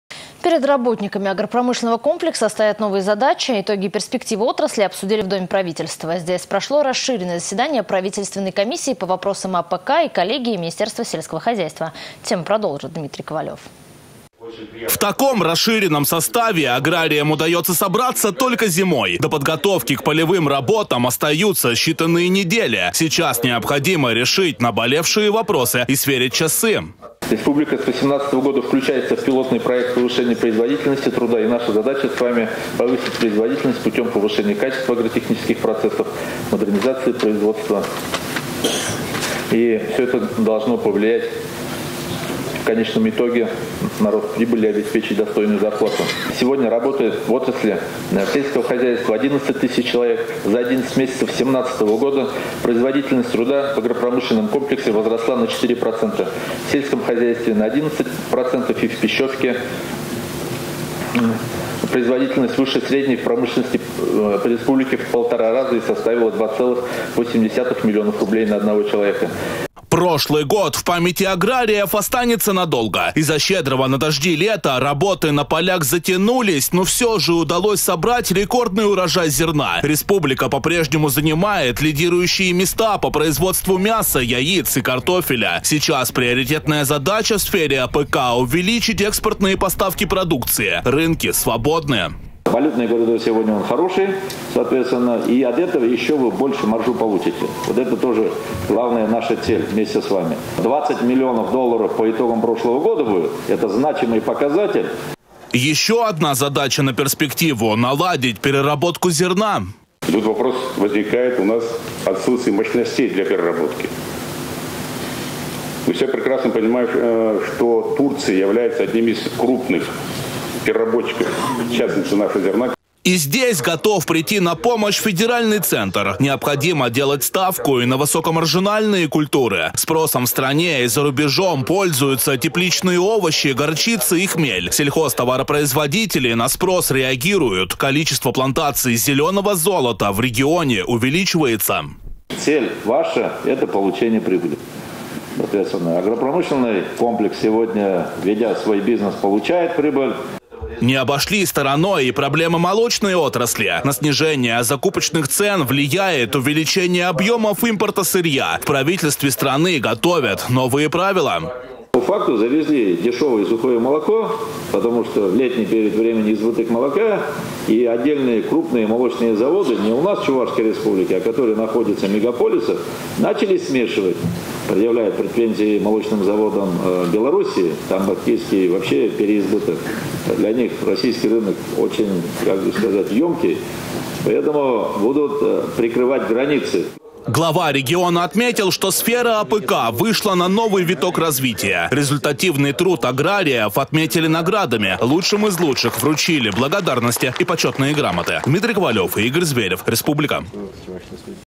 Перед работниками агропромышленного комплекса стоят новые задачи.
Итоги и перспективы отрасли обсудили в Доме правительства. Здесь прошло расширенное заседание Правительственной комиссии по вопросам АПК и коллегии министерства сельского хозяйства.